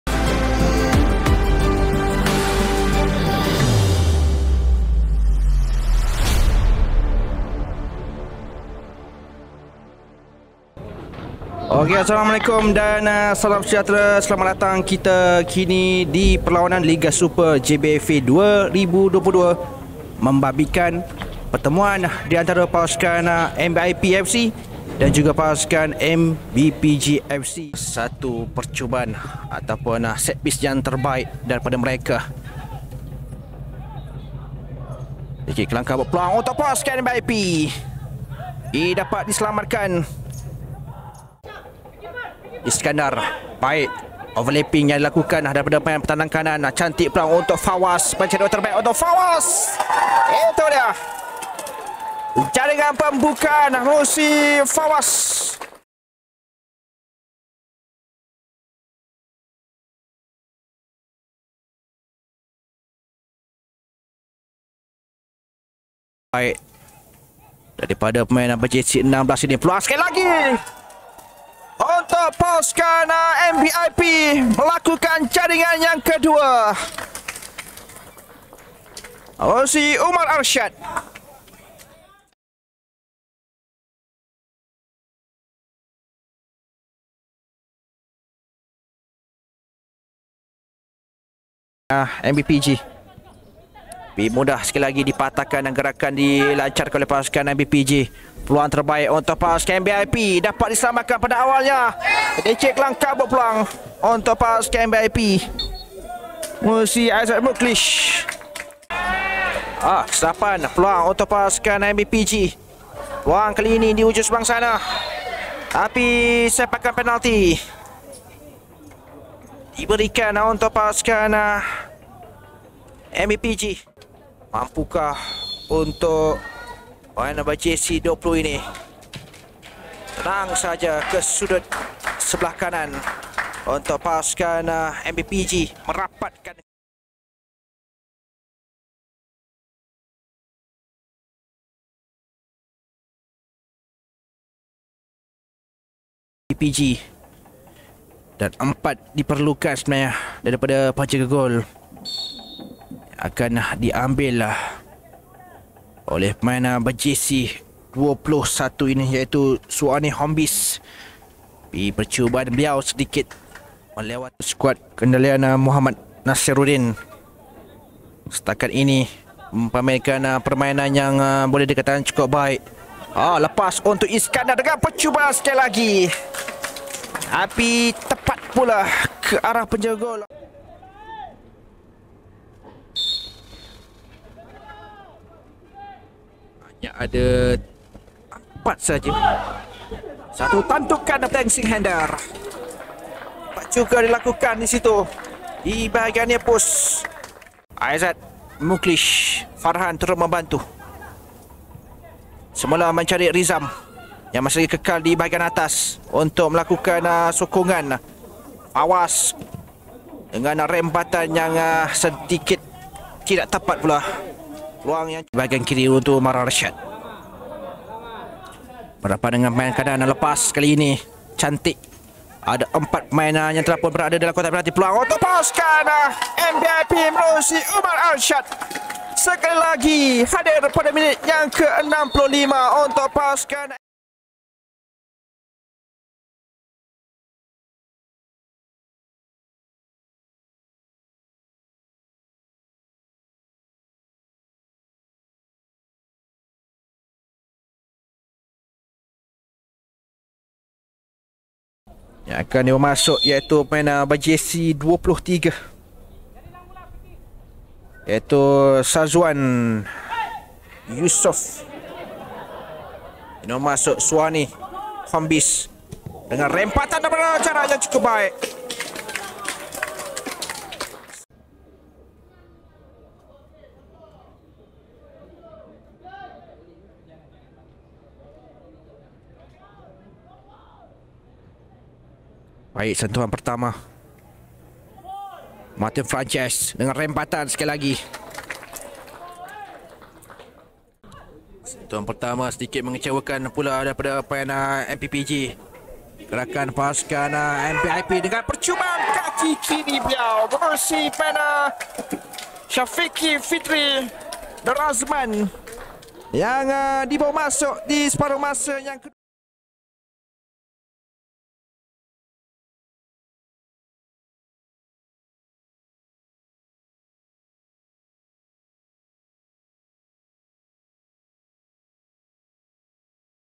MBIP FC 4 - 1 MBPG FC | Liga Super JBFA 2022 | ZA Studios
Tamat perlawanan Liga super JBFA 2022 diantara MBIP FC dan MBPG FC perlawanan yang cukup sengit memberikan 3 mata penuh untuk pasukan MBIP di aksi pembukaan.